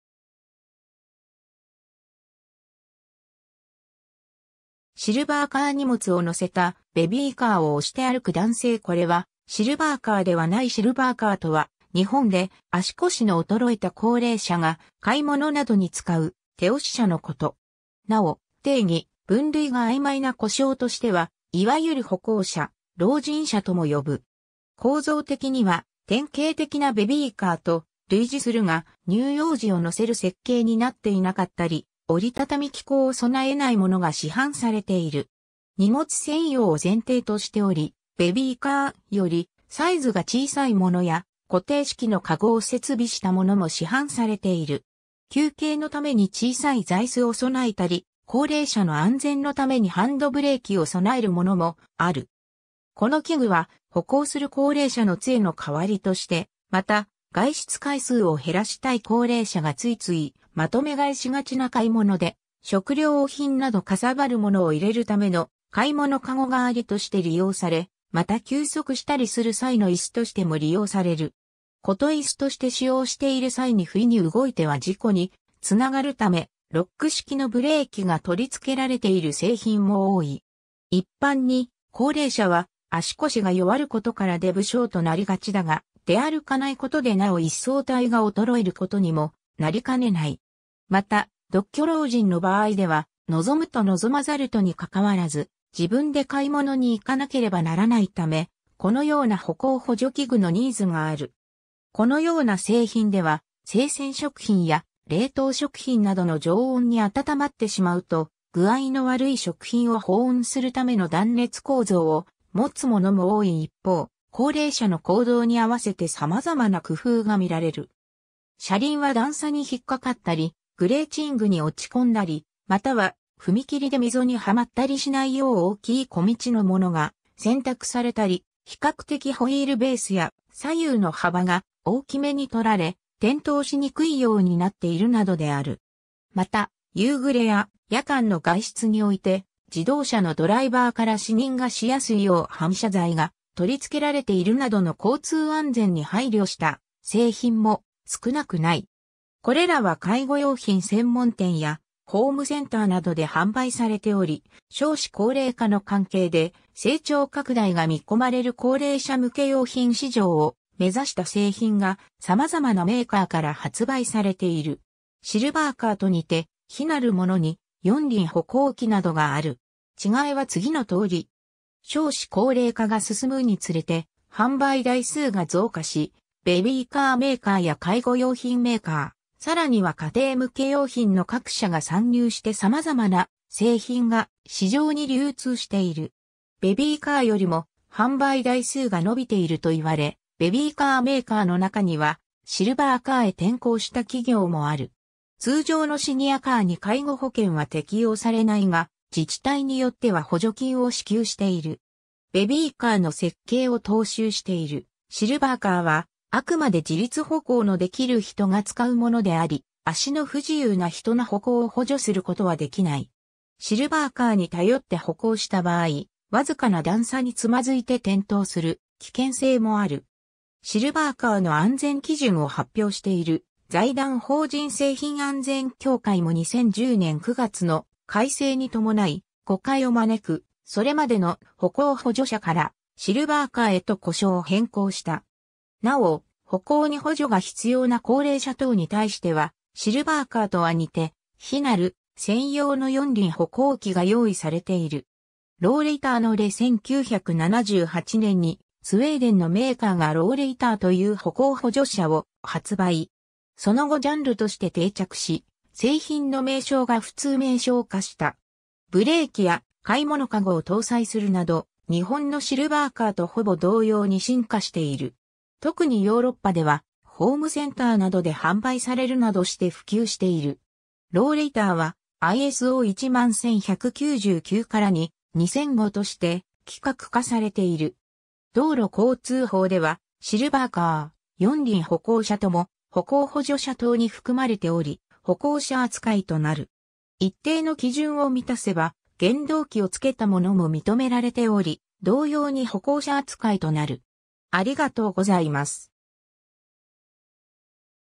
シルバーカー
シルバーカー （参考）荷物を載せたベビーカーを押して歩く男性 ※これはシルバーカーではない シルバーカーとは、日本で足腰の衰えた高齢者が買い物などに使う手押し車の事。
なお、定義、分類が曖昧な呼称としてはいわゆる歩行車、老人車とも呼ぶ。
 構造的には典型的なベビーカーと類似するが、乳幼児を載せる設計になっていなかったり、折り畳み機構を備えないものが市販されている（備えるものもある）。
荷物専用を前提としており、ベビーカーよりサイズが小さいものや、固定式のカゴを設備したものも市販されている。
 休憩のために小さい座椅子を備えたり、高齢者の安全のためにハンドブレーキを備えるものもある（ベビーカーは通例ストッパーだけである）。
 この器具は、歩行する高齢者の杖の代わりとして、また外出回数を減らしたい高齢者がついついまとめ買いしがちな買い物で、食料品などかさばるものを入れるための買い物篭代わりとして利用され、また休息したりする際の椅子としても利用される。
こと椅子として使用している際に不意に動いては事故につながるため、ロック式のブレーキが取り付けられている製品も多い。
 一般に高齢者は足腰が弱ることから出不精となりがちだが、出歩かないことでなお一層体が衰えることにもなりかねない。
また独居老人の場合では望むと望まざるとに関わらず、自分で買物に行かなければならないため、このような歩行補助器具のニーズがある。
 このような製品では、生鮮食品や冷凍食品などの常温に温まってしまうと具合の悪い食品を保温するための断熱構造をもつものも多い一方、高齢者の行動にあわせて様々な工夫が見られる。
車輪は段差に引っかかったりグレーチングに落ち込んだり、または踏切で溝にはまったりしないよう大きい径のものが選択されたり、比較的ホイールベースや左右の幅が大きめに取られ、転倒しにくいようになっているなどである。
また、夕暮れや夜間の外出において自動車のドライバーから視認がしやすいよう反射材が取り付けられているなどの交通安全に配慮した製品も少なくない。
 これらは介護用品専門店やホームセンターなどで販売されており、少子高齢化の関係で成長拡大が見込まれる高齢者向け用品市場を目指した製品が、様々なメーカーから発売されている（後述）。
 シルバーカーと似て非なるものに、四輪歩行器（四輪歩行車）などがある。
違いは次の通り。
 少子高齢化が進むにつれて販売台数が増加し、ベビーカーメーカーや介護用品メーカー、さらには家庭向け用品の各社が参入して様々な製品が市場に流通している。
ベビーカーよりも販売台数が伸びていると言われ、ベビーカーメーカーの中にはシルバーカーへ転向した企業もある。
通常のシニアカーに介護保険は適用されないが、自治体によっては補助金を支給している。
 ベビーカーの設計を踏襲しているシルバーカーは、あくまで自立歩行のできる人が使うものであり、足の不自由な人の歩行を補助する事はできない。
シルバーカーに頼って歩行した場合、僅かな段差につまずいて転倒する危険性もある。
シルバーカーの安全基準を発表している財団法人製品安全協会も2010年9月の改正に伴い、誤解を招くそれまでの「歩行補助車」から「シルバーカー」へと呼称を変更した。
なお、歩行に補助が必要な高齢者等に対しては、シルバーカーとは似て非なる専用の四輪歩行器が用意されている。
 Rollatorの例 1978年にスウェーデンのメーカーがRollatorと言う歩行補助車を発売。
その後ジャンルとして定着し、製品の名称が普通名称化した。
ブレーキや買い物かごを搭載するなど、日本のシルバーカーとほぼ同様に進化している。
特にヨーロッパではホームセンターなどで販売されるなどして普及している。
 RollatorはISO 11199-2:2005として規格化されている。
 道路交通法ではシルバーカー、四輪歩行車とも「歩行補助車等」に含まれており、歩行者扱いとなる。
一定の基準を満たせば、原動機を付けたものも認められており、同様に歩行者扱いとなる。